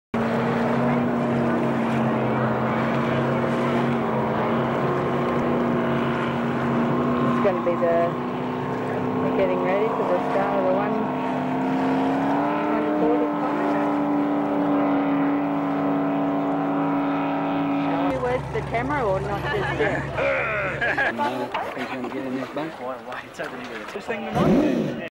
Frantic
Frantic before the start of the 1997 Australian Titles 175hp class at Jindabine